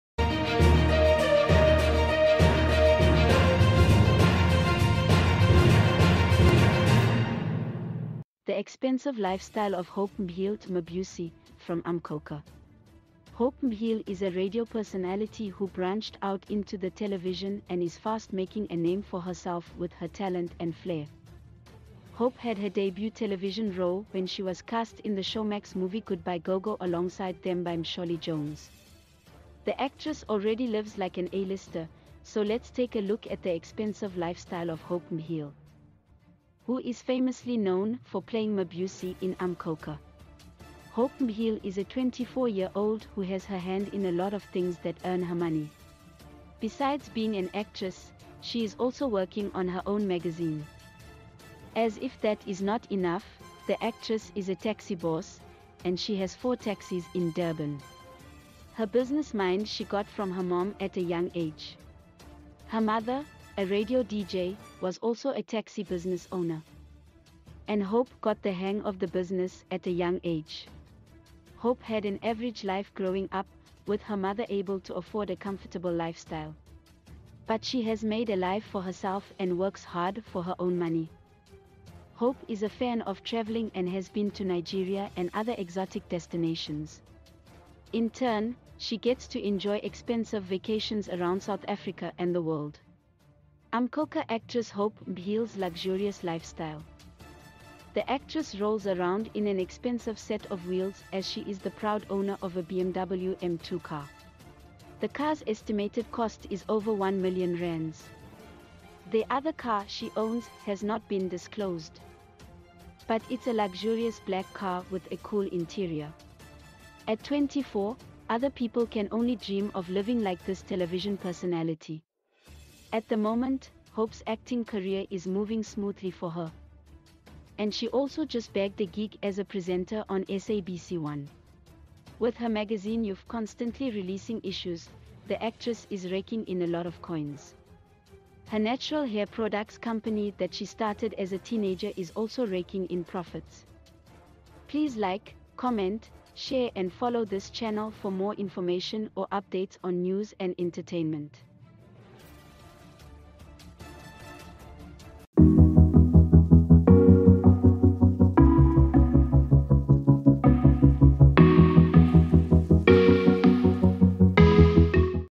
The expensive lifestyle of Hope Mbhele ‘Mabusi’ from Umkhokha
The expensive lifestyle of Hope Mbhele ‘Mabusi’ from Umkhokha - Hope Mbhele is a radio personality who branched out into the Television and is fast making a name for herself with her talent and flair. Hope had her debut Television role when she was cast in the Showmax movie Goodbye Gogo alongside Thembi Mtshali Jones. The actress already lives like an A-lister, so let’s take a look at the expensive lifestyle of Hope Mbhele, who is famously known for playing Mabusi in Umkhokha.

Hope Mbhele is a twenty-four year old who has her hand in a lot of things that earn her money. Besides being an actress, she is also working on her own magazine. As if that is not enough, the actress is a taxi boss, and she has four taxis in Durban. Her business mind she got from her mom at a young age. Her mother, a radio DJ, was also a taxi business owner, and Hope got the hang of the business at a young age.